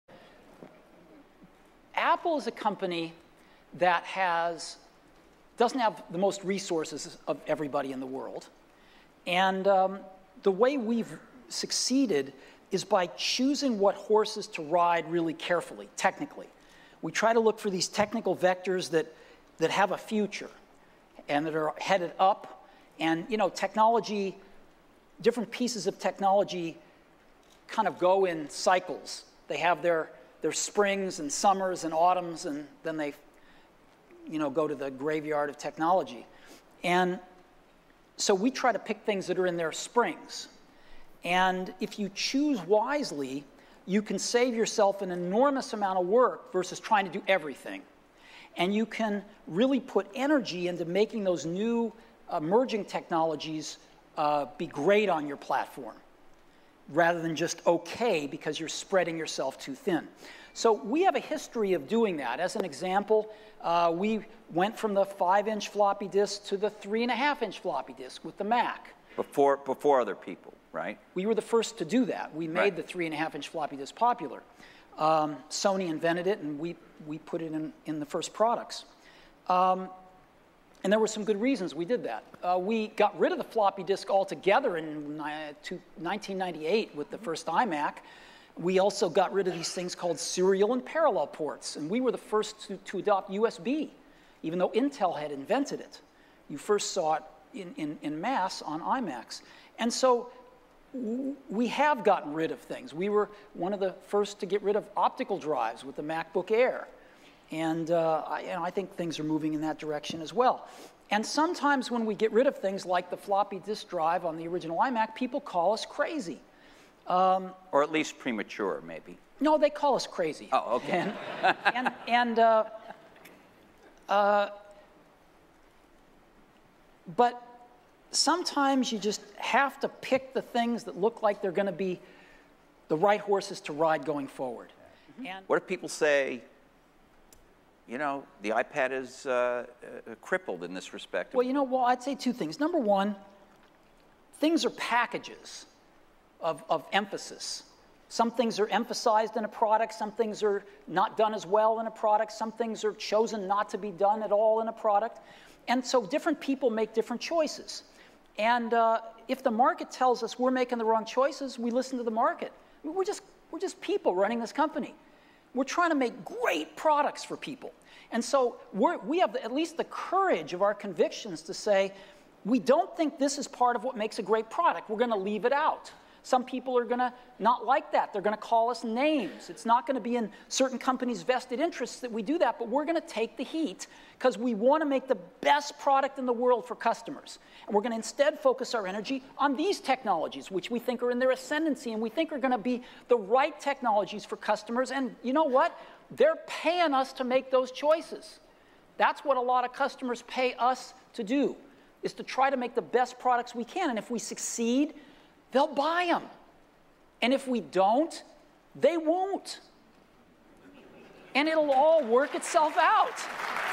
Steve Jobs - Courage
This is a clip from the D8 Conference, recorded in 2010.
Steve Jobs is talking about the courage it takes to remove certain pieces of technology from Apple products. This happened after the iPad was introduced without support for Flash, just as the iPhone, back in 2007.

This clip adds some perspective into the debate of Apple's new AirPod and the decision to remove the traditional analog audio connector from the iPhone 7. This kind of decision is not new to Apple.

The interview occurred 6 years ago, and Steve Jobs was already using the word "courage" to explain why the company does things the way it does.